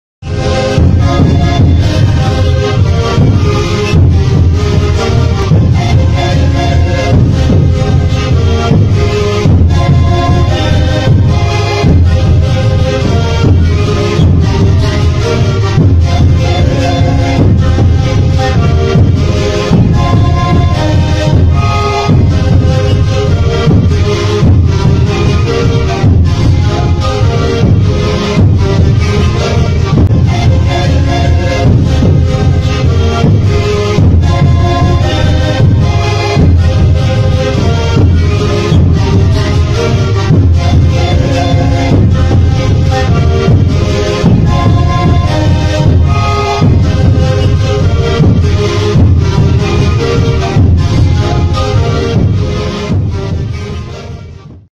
💙 SIKURIS INTERCONTINENTALES AYMARAS DE HUANCANÉ
Ensayo de los Sikuris Intercontinentales Aymaras de Huancané.